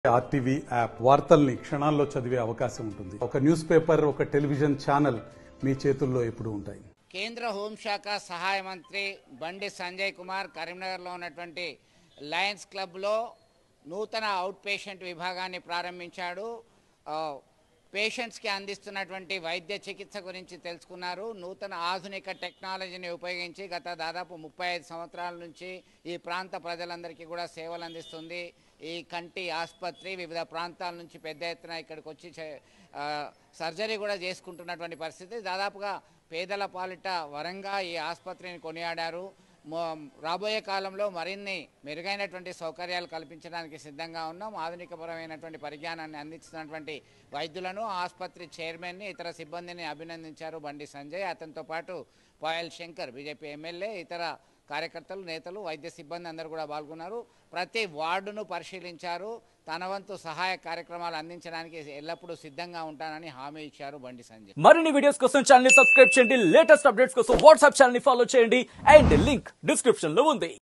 Bandi Sanjay Inaugurated Newly Established outpatient ward | రేకుర్తి ఐ హాస్పిటల్ కు ఎంపీ బండి సంజయ్
Bandi Sanjay Inaugurated Newly Established outpatient ward | రేకుర్తి ఐ హాస్పిటల్ కు ఎంపీ బండి సంజయ్ | RTV